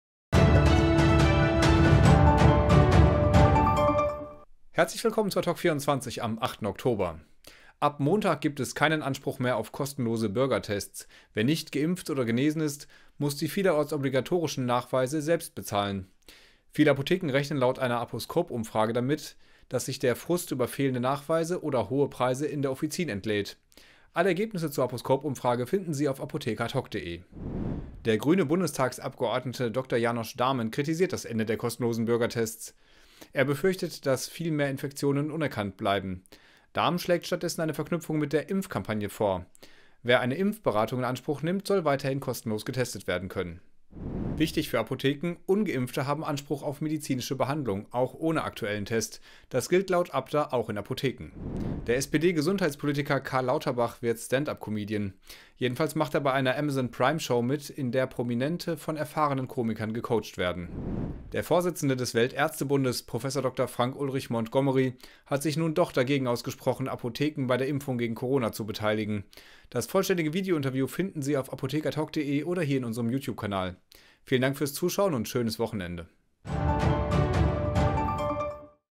Apotheken fürchten Test-Frust / Dahmen: Gratistests mit Impfberatung / Lauterbach als Comedian
Ab Montag gibt es keinen Anspruch mehr auf kostenlose Bürgertests. Viele Apotheken rechnen damit, dass sich der Frust über fehlende Nachweise und teure Preise in der Offizin entladen könnte. / Der grüne Bundestagsabgeordnete Dr. Janosch Dahmen schlägt eine Verknüpfung mit der Impfkampagne vor. Wer eine Impfberatung annimmt, sollte weiter gratis getestet werden. / Wichtig für Apotheken: Ungeimpfte haben Anspruch auf eine ärztliche Behandlung – auch ohne aktuellen Corona-Test. Für Apotheken gilt laut Abda dasselbe. / Der SPD-Gesundheitspolitiker Karl Lauterbach macht er bei einer Show von Amazon Prime mit, in der Prominente von erfahrenen Komikern gecoacht werden. / Der Vorsitzende des Weltärztebundes Professor Dr. Frank Ulrich Montgomery, ist gegen Beteiligung der Apotheken an der Impfkampagne.
Das kompetente Redaktionsteam aus Journalisten und Pharmazeuten von APOTHEKE ADHOC berichtet über die jüngsten Entwicklungen in Gesundheitspolitik, Pharma- und Apothekenmarkt, Pharmazie und Medizin.